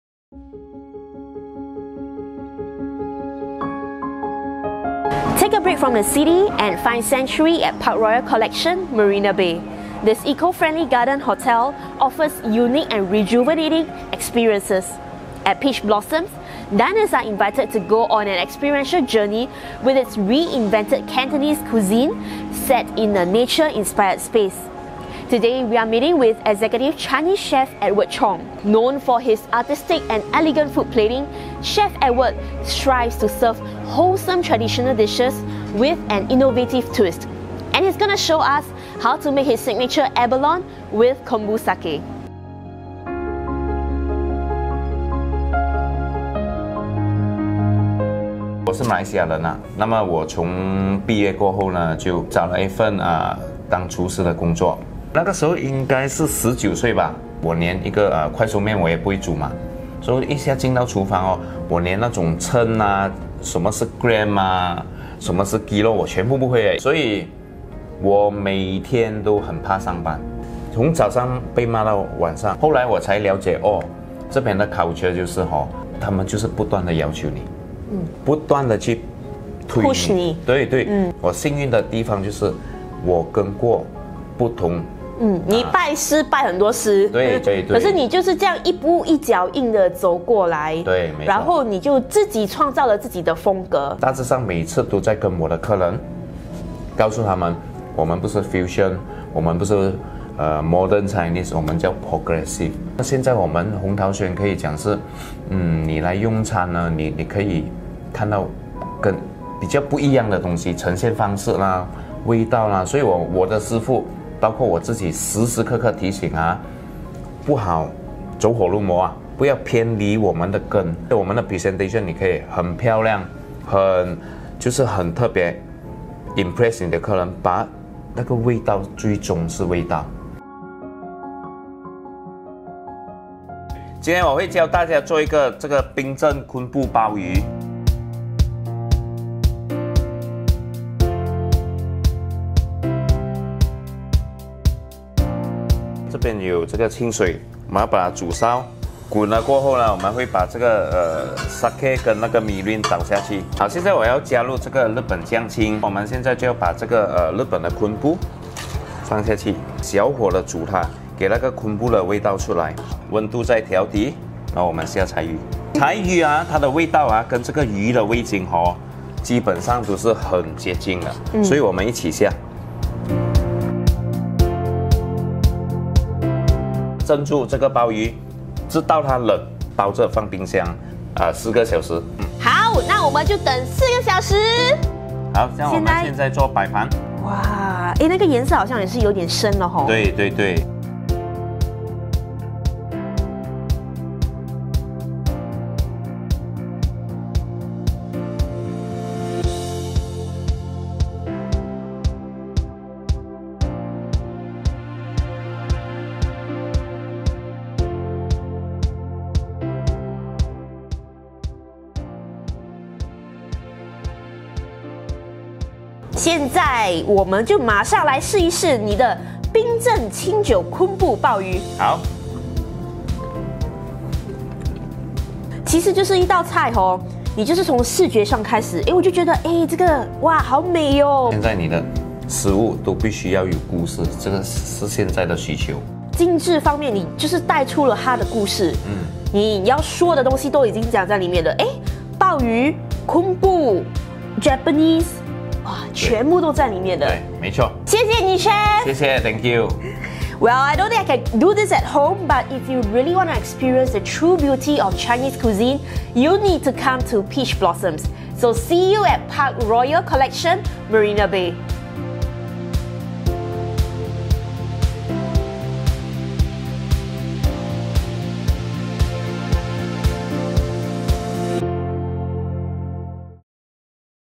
Sample Chef Edward Chong's Signature Abalone with Konbu Sake at Peach Blossoms
*SPECIAL* Redeem a complimentary Abalone with Konbu Sake with a minimum spend of $100.

Known for his elegant and artistic food plating, Executive Chinese Chef Edward Chong from Peach Blossoms at PARKROYAL COLLECTION Marina Bay, Singapore believes in giving new life to traditonal Cantonese dishes.

With views of the iconic Marina Bay, Peach Blossoms offers a warm and inviting ambience for a culinary discovery into a whole new world of Chinese dining.